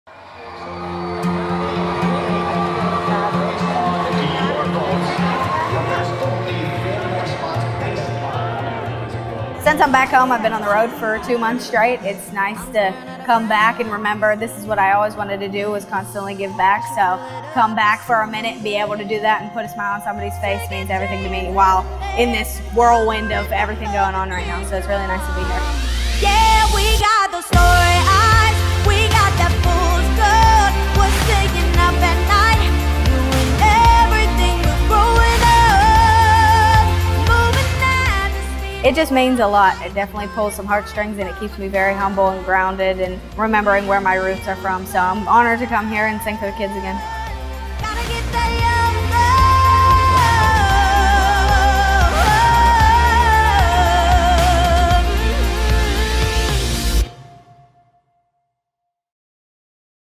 Gabby Barrett Visits UPMC Children's Hospital of Pittsburgh
Gabby Barrett, who finished third in season 16 of American Idol, visited UPMC Children’s and performed for our patients and families! We can’t thank her enough for her support!